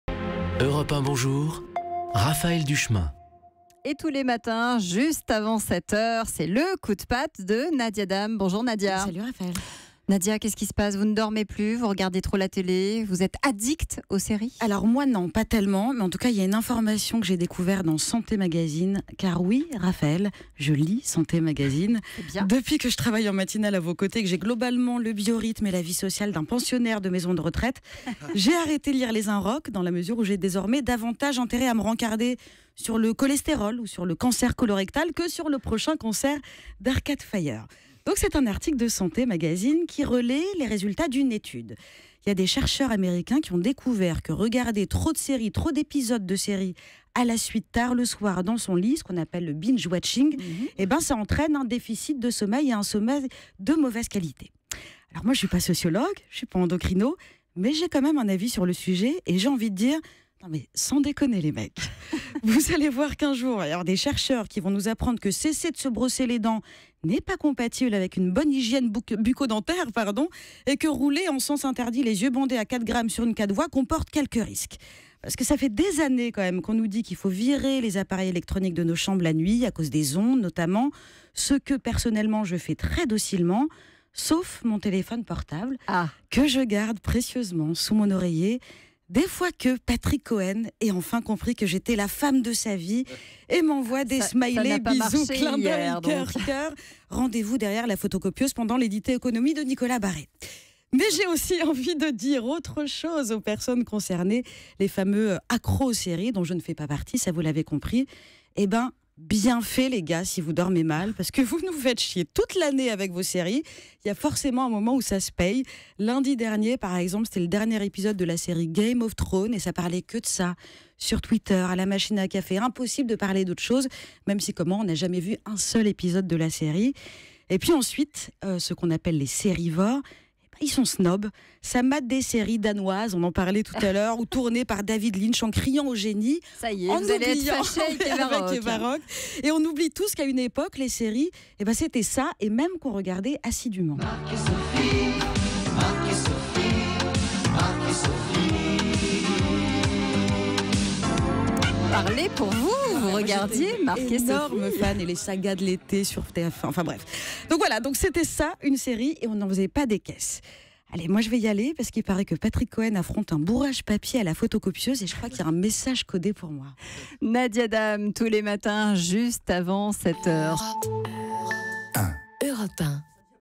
Regarder trop de séries le soir nuit au sommeil, quelle découverte !
Chaque jour, Nadia Daam vous présente son coup de patte personnel. En ce jeudi, la chroniqueuse revient sur une étude sur le "binge watching" qui nuit au sommeil.